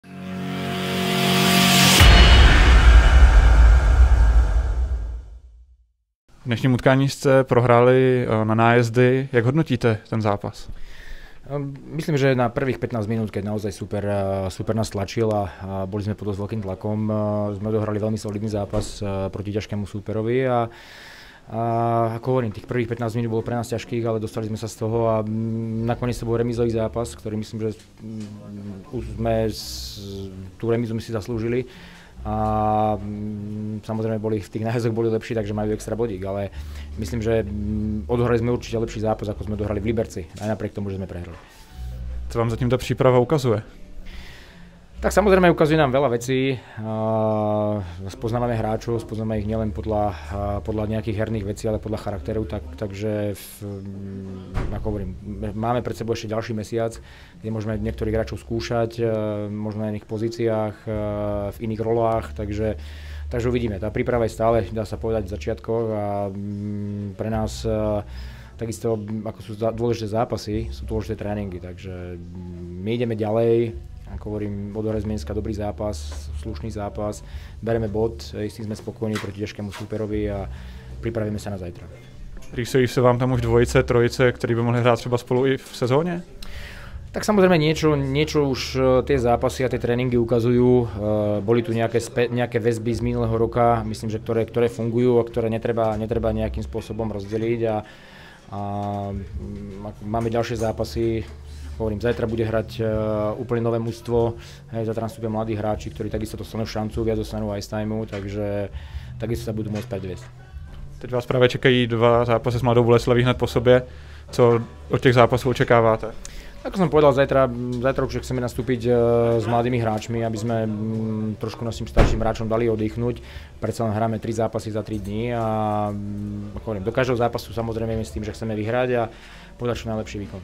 V. Országh: Kvalitní soupeř, bod bereme
Hlavní trenér Vervy Vladimír Országh hodnotí utkání s Libercem, ve kterém jeho svěřenci prohráli po samostatných nájezdech.